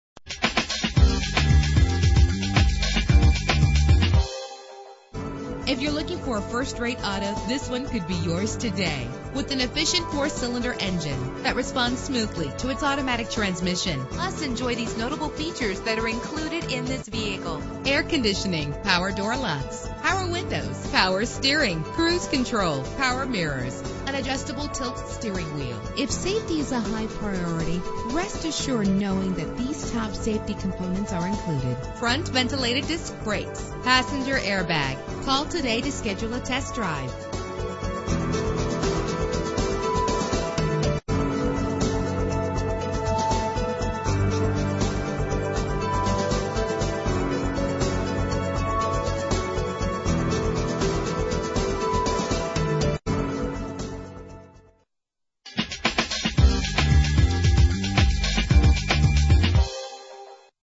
2000 Nissan Altima - Dahlonega GA
Year: 2000
Make: Nissan
Model: Altima
Trim: GLE
Engine: 2.4 liter inline 4 cylinder 16 valve
Transmission: 4-Speed Automatic
Color: Platinum Clearcoat Metallic
Mileage: 135203
Address:
44 Lumpkin County Pkwy
Dahlonega, GA 30533